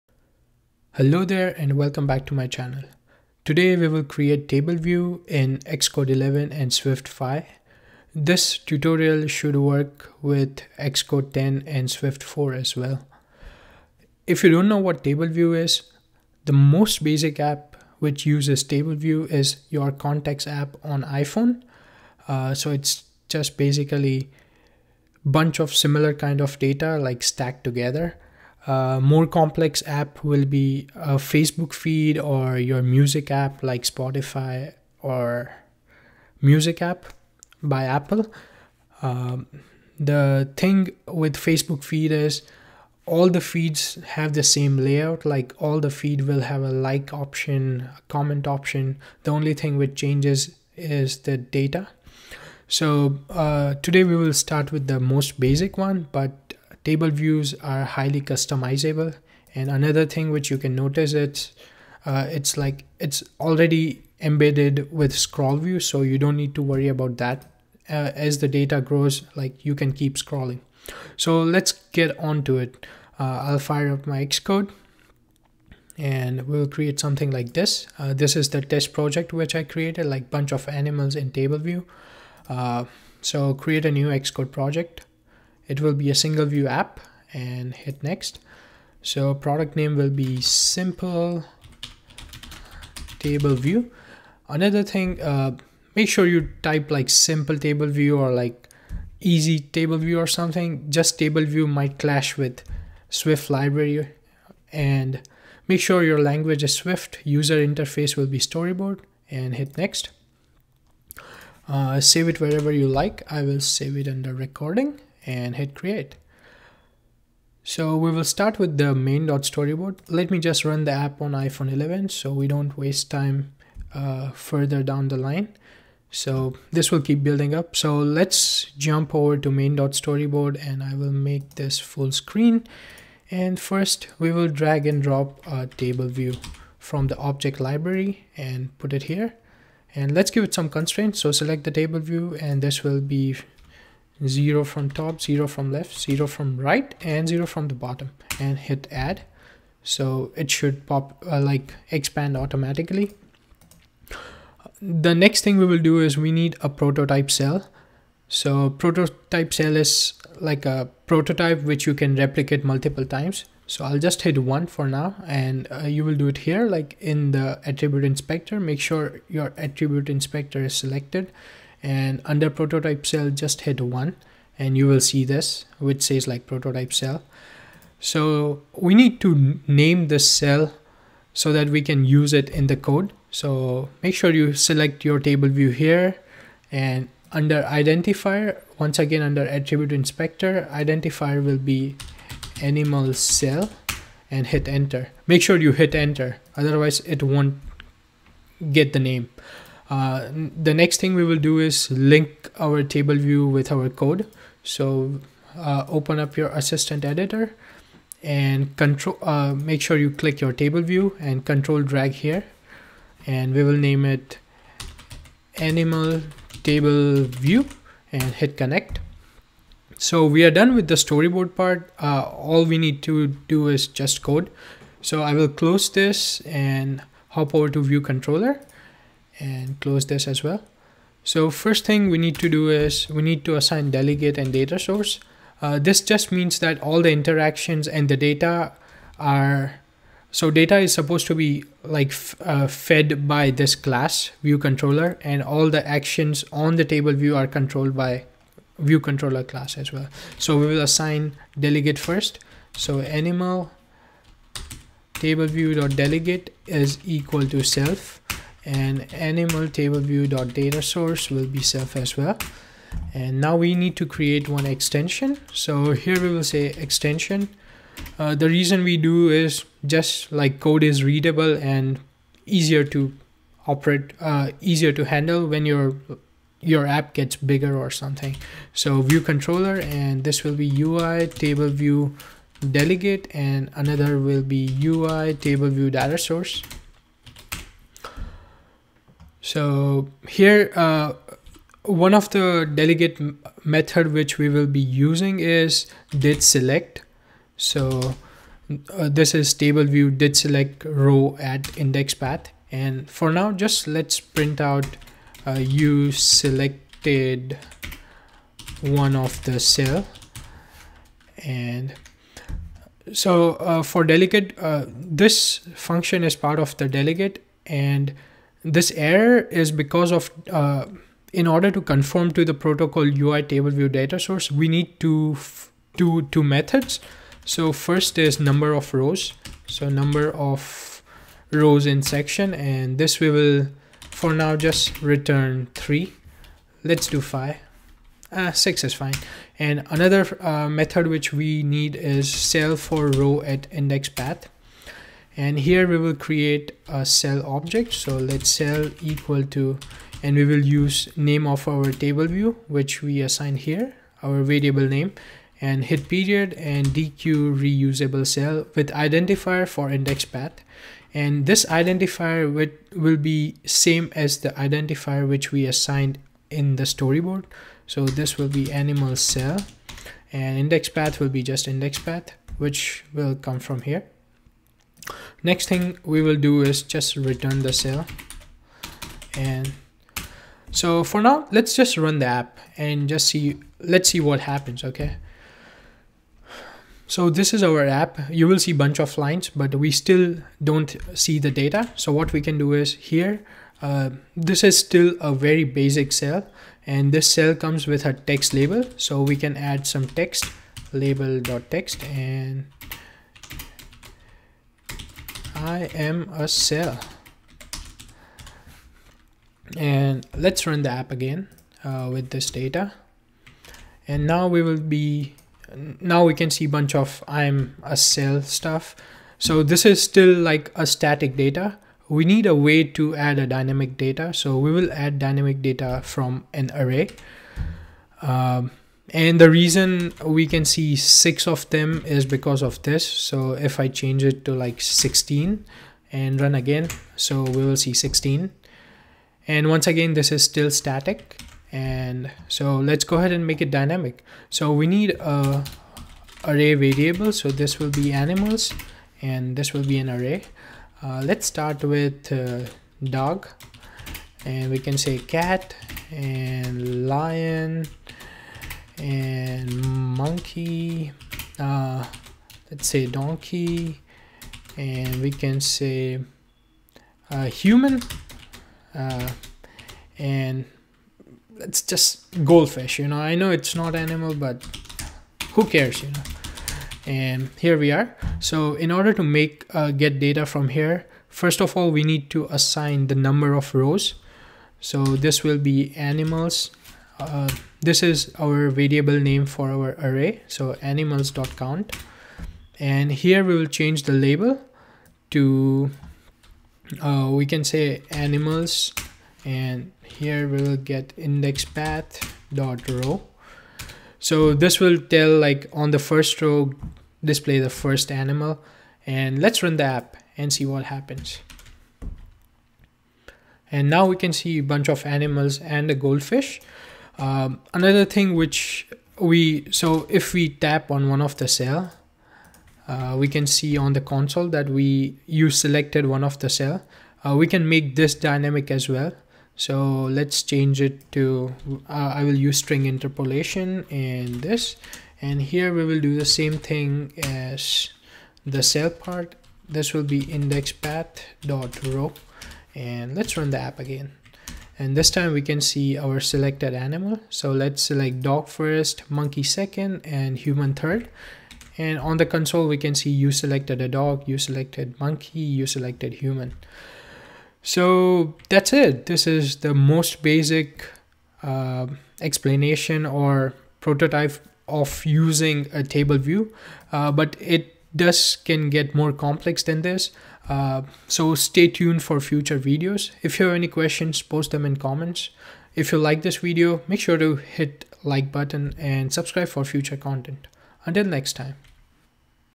How to Create Simple TableView in Xcode 11 (Swift 5)
In this video I will show you simple way of creating table view in Xcode 11 and Swift 5. This tutorial is not limited to Xcode 11 or Swift 5. This tutorial will work for previous versions of Xcode and Swift. This tutorial will work with Xcode 10 and Swift 4.

Swift 5.1.3/ Xcode 11.4.1
My mac version macOS Catalina